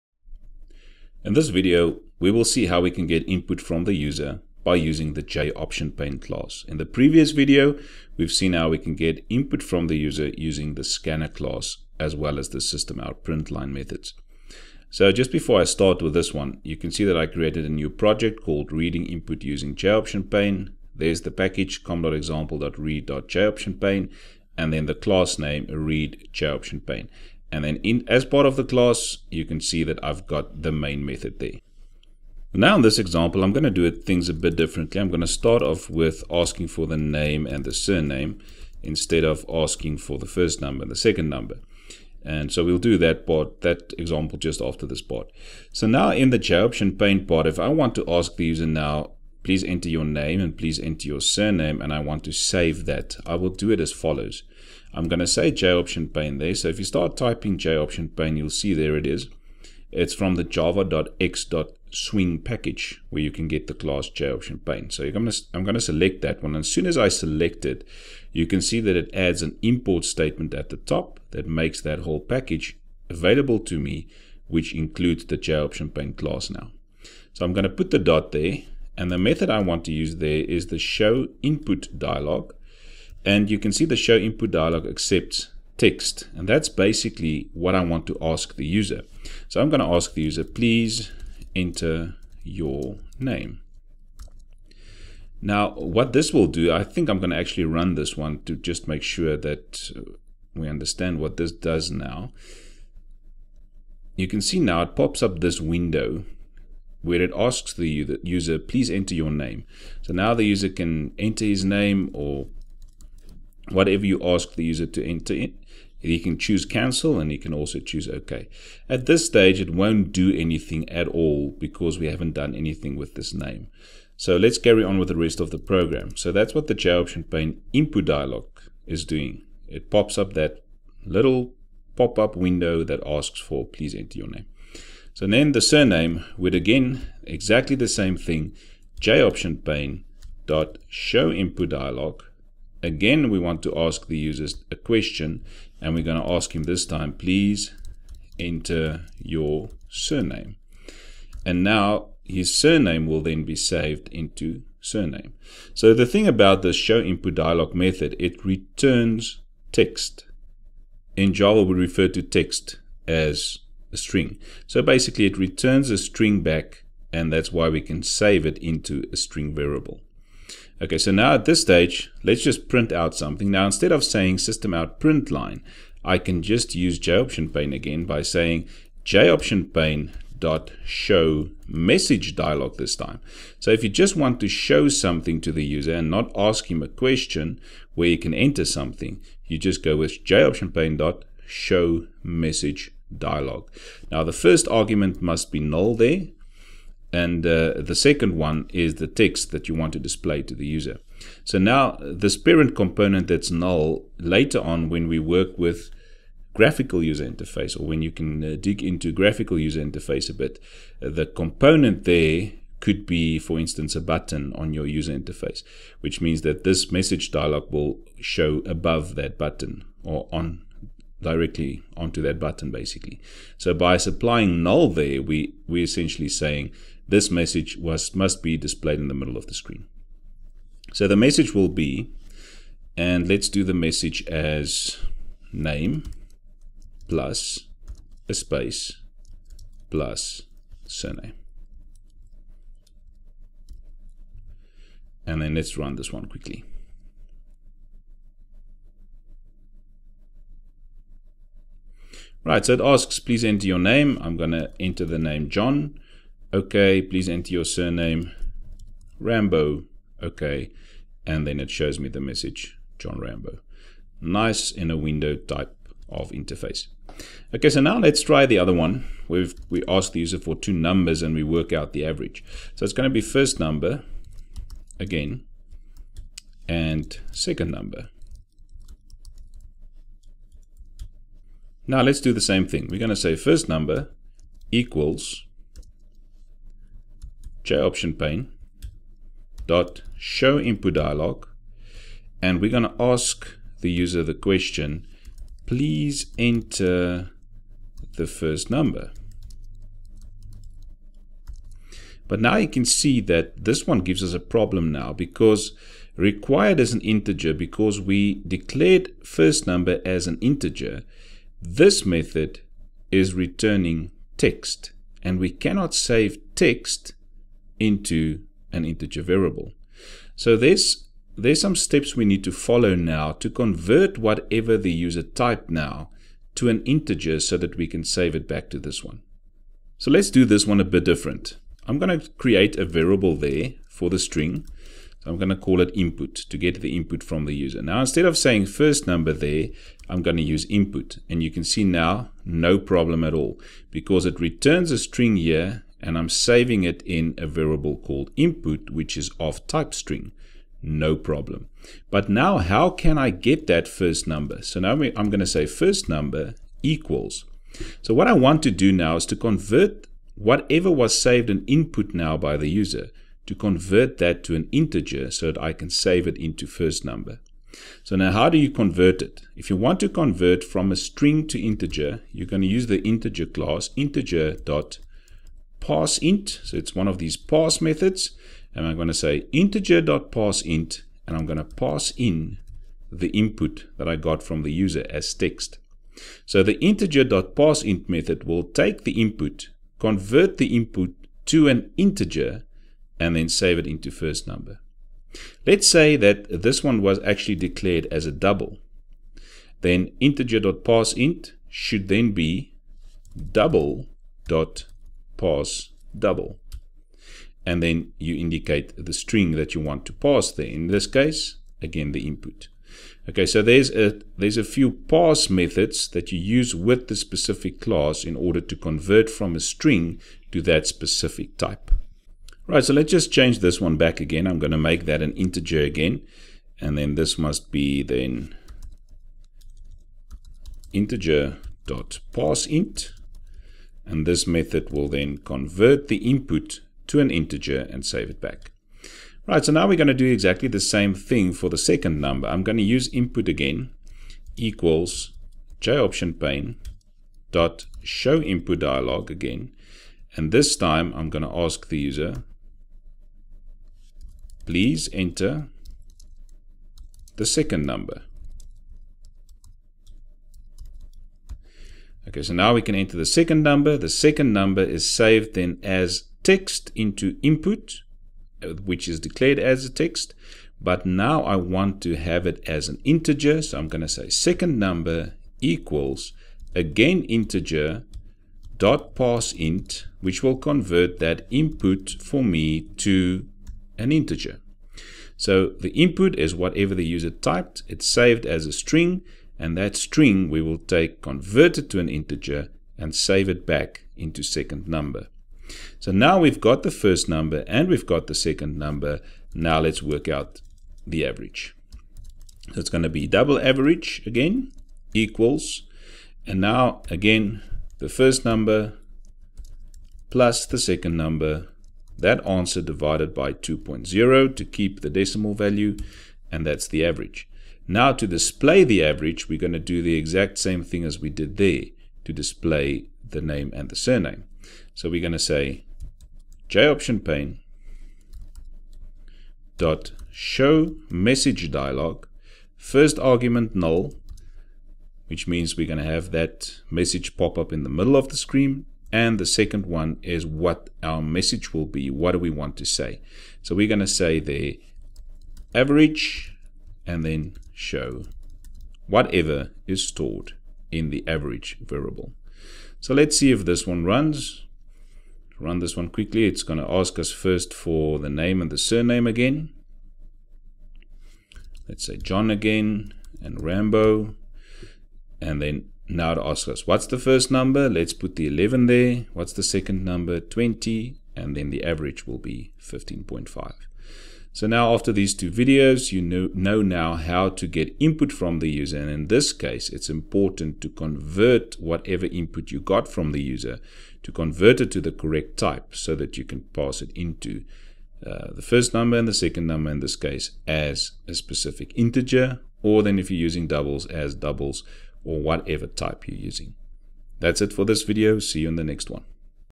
5. Interacting with the user using the JOptionPane class
In this video I explain how to use the JOptionPane class in Java to read input from the user.